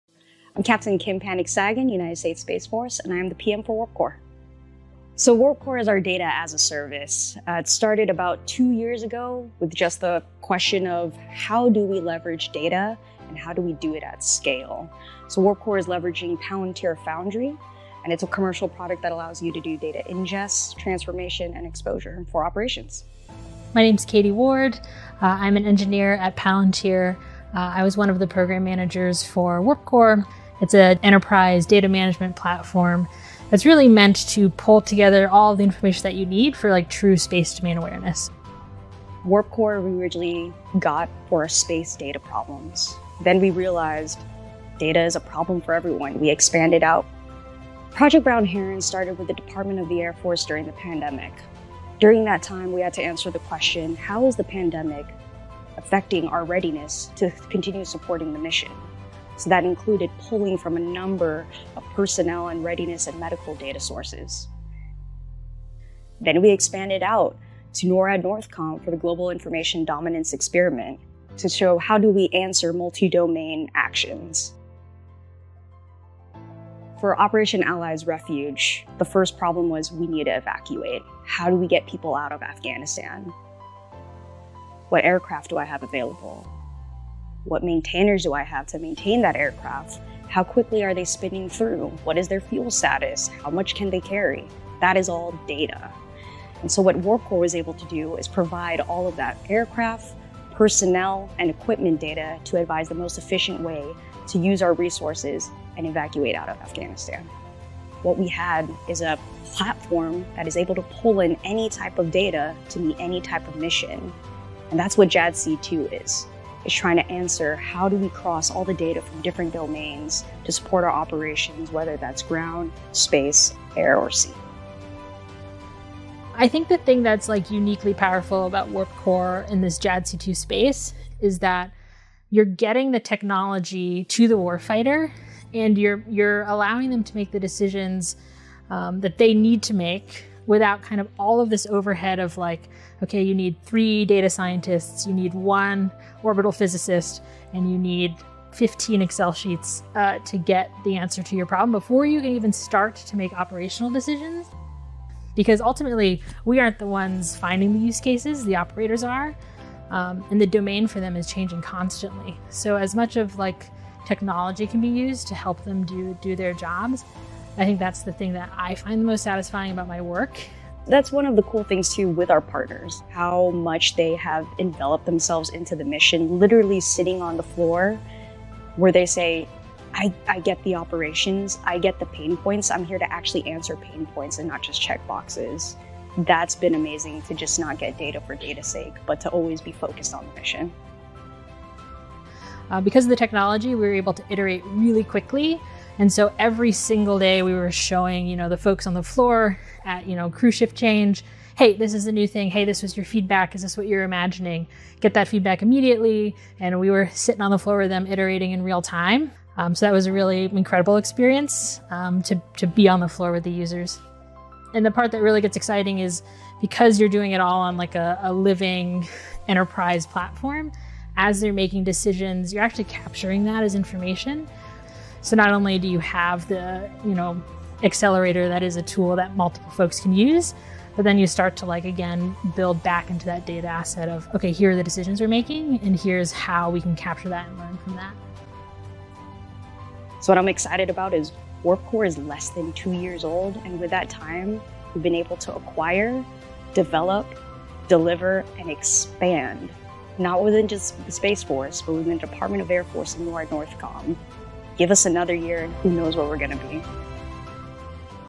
Palantir's Partnership With US Space Force
Palantir has secured a number of contracts from the US Space Force, a display as to how highly regarded this company is by the US Government. Here's an insight into what they get up to.

Bite-sized videos to digest a complex company.